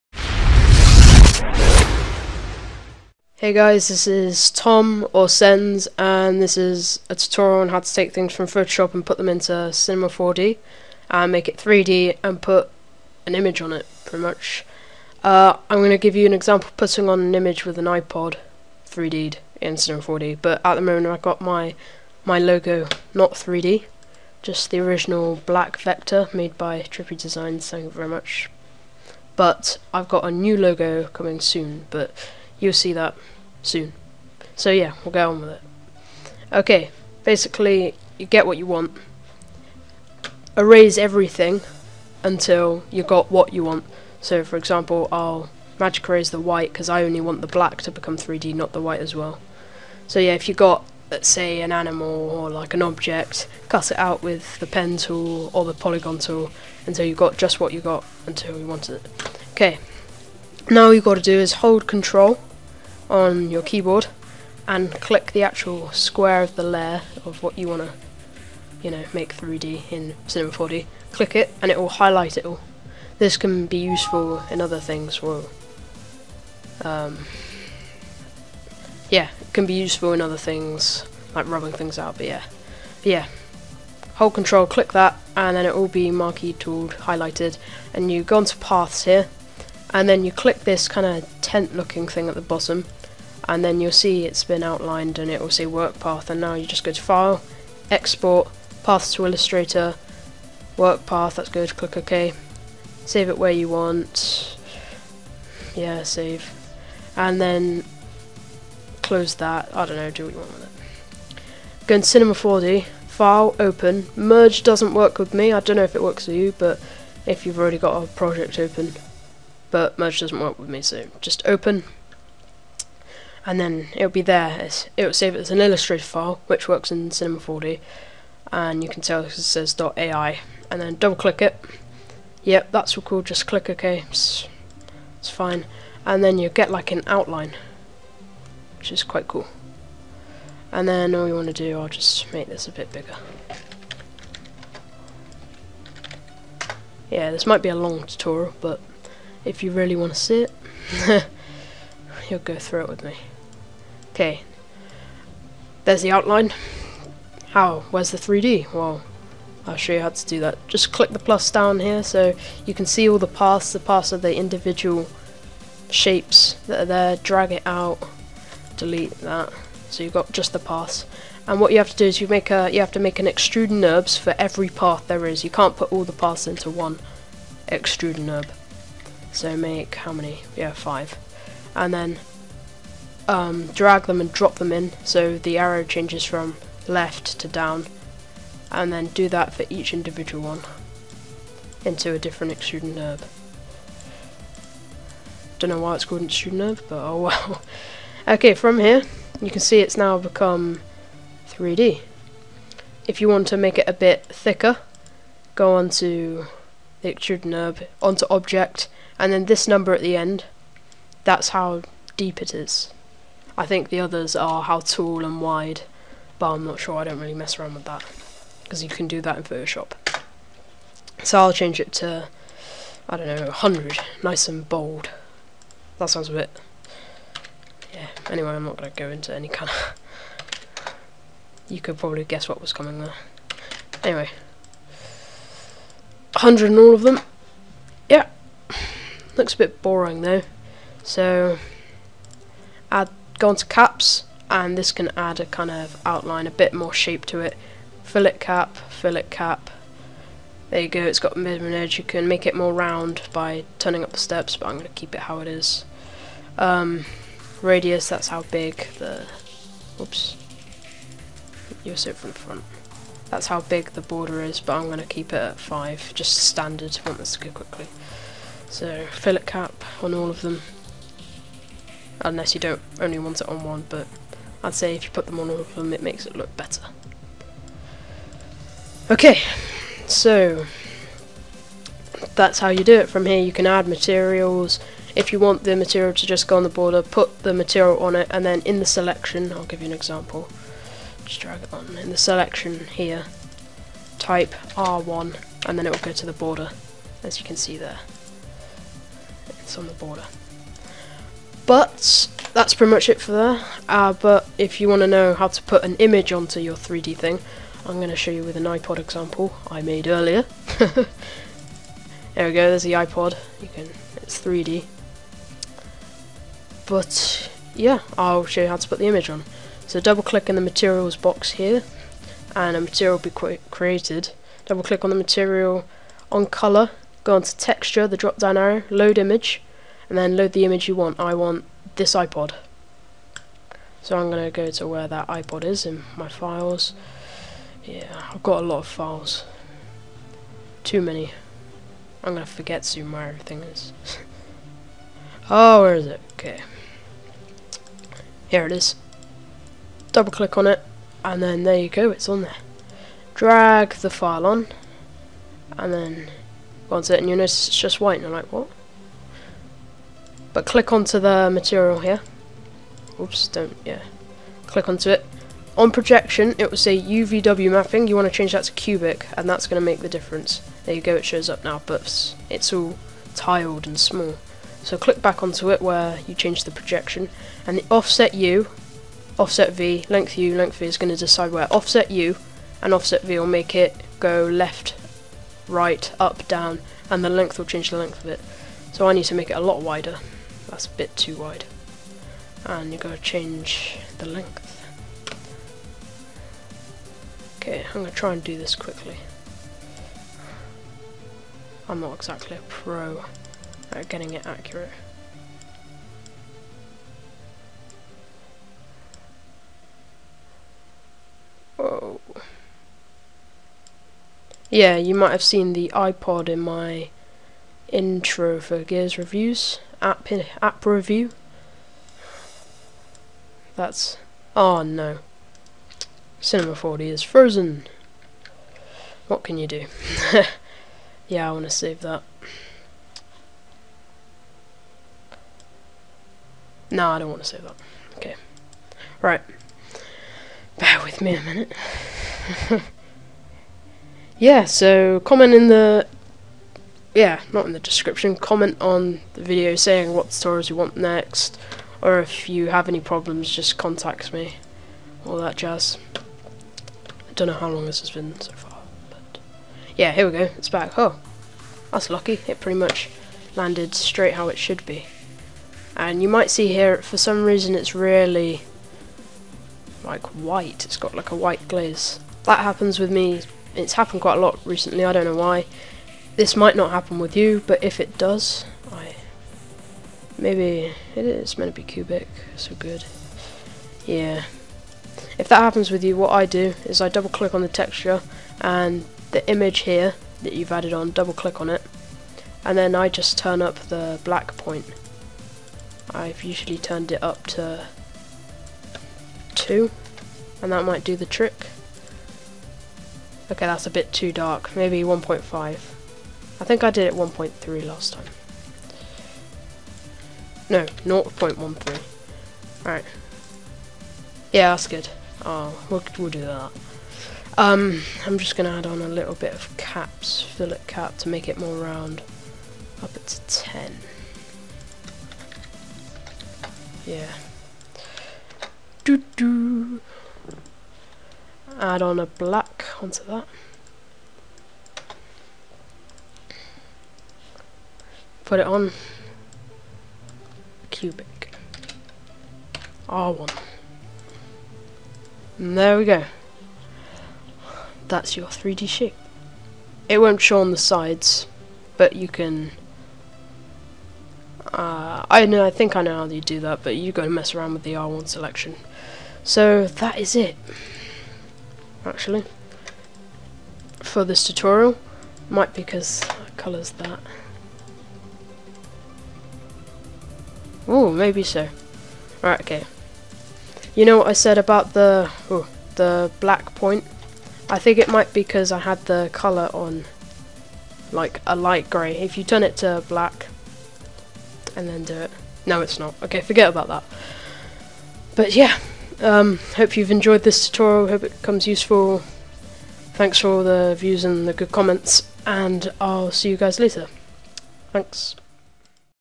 Tutorial - Photoshop to Cinema 4d (3d-ing) and applying an image
Publicised from private, RIP comments sections :-(

My second full tutorial on taking things from photoshop, eg. an image, and making it 3d in cinema 4d and also applying an image onto it :D hope you guys enjoy and find this useful, thanks :P